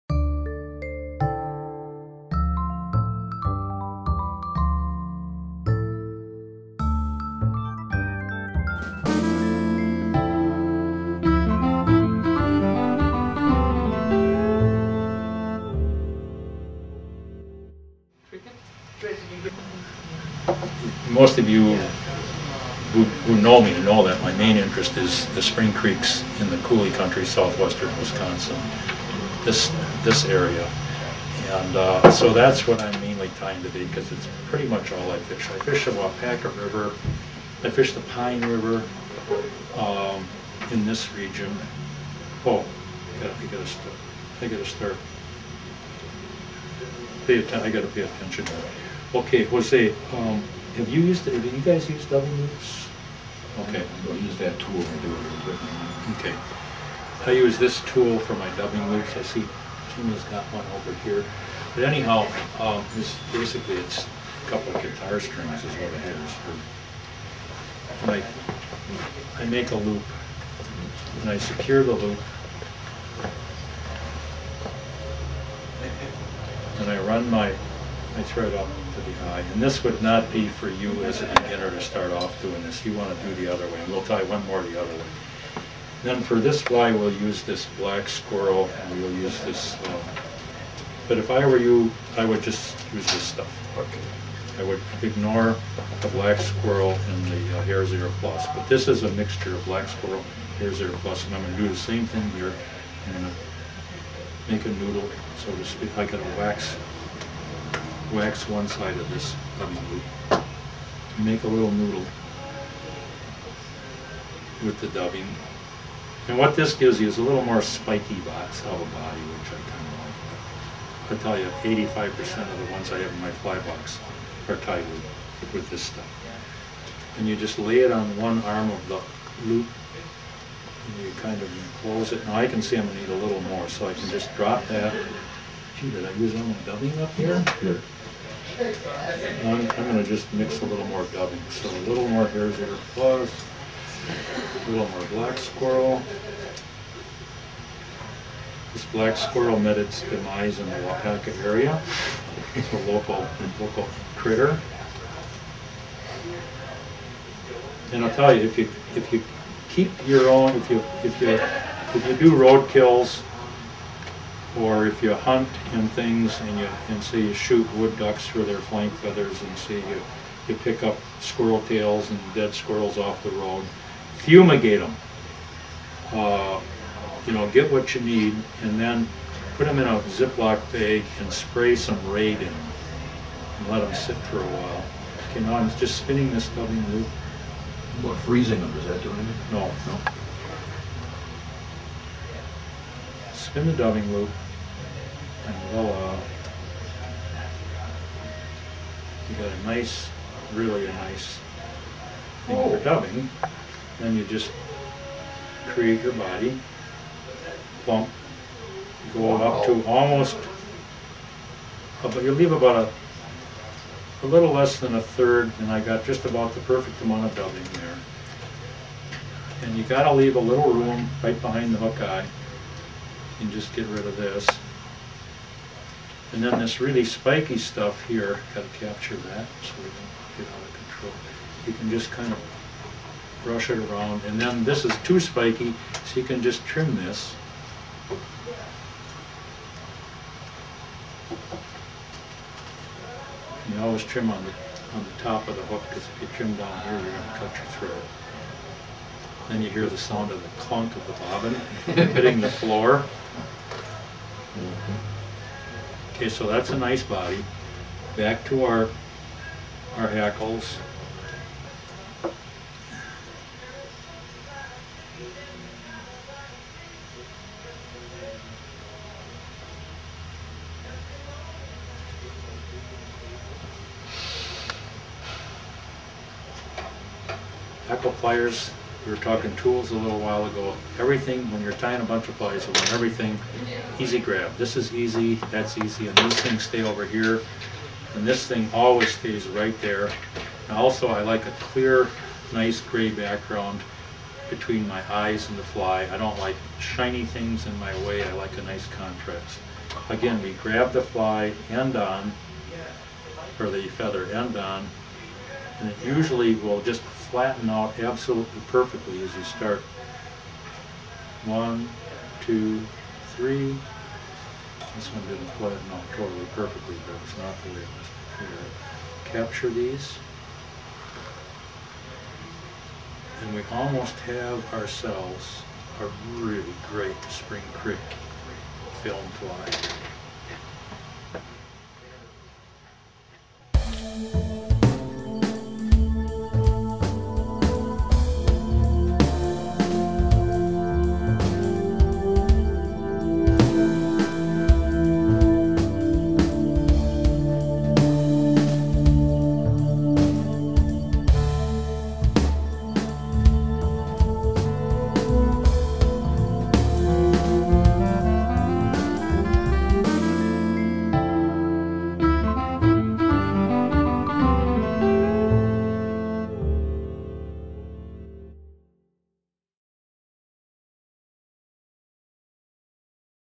Fly tying lesson w/Ross Mueller -Tying the Film Fly
Tight Lines Fly Fishing Co. proudly presents Ross Mueller Tying his Film Fly. This is a great black caddis pattern. Enjoy